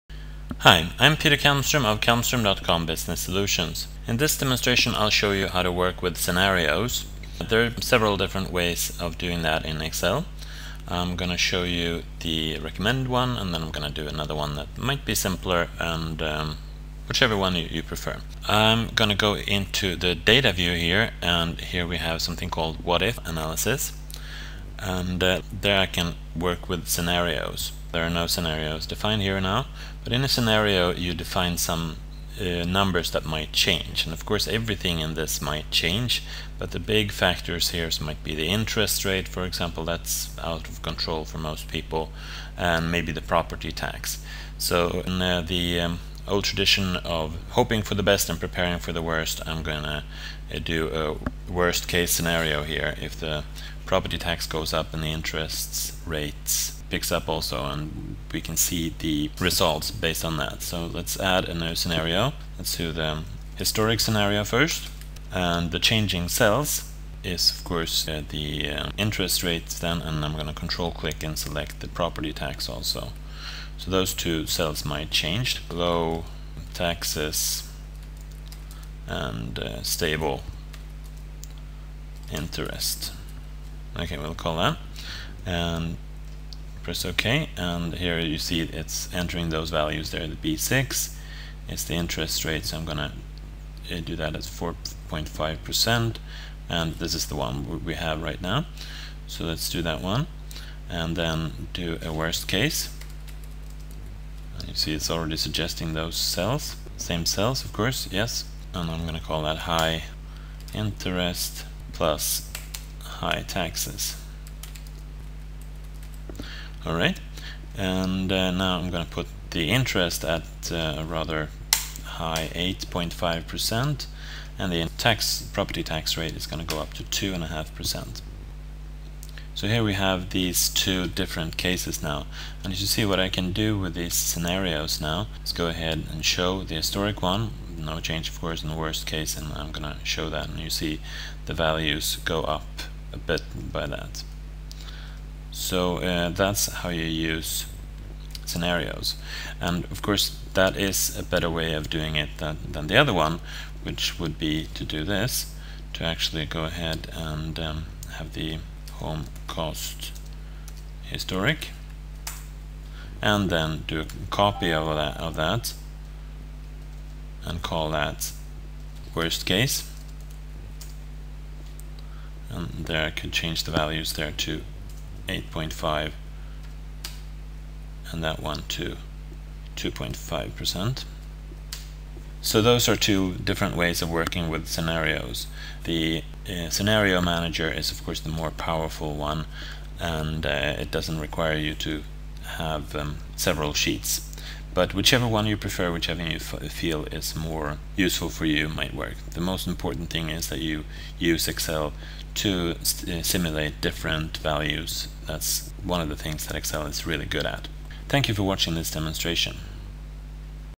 Excel Scenarios
Use the Excel Scenario Manager to show how data changes in different scenarios. Peter Kalmstrom explains step by step how to do it the recommended way - and how you also can do it.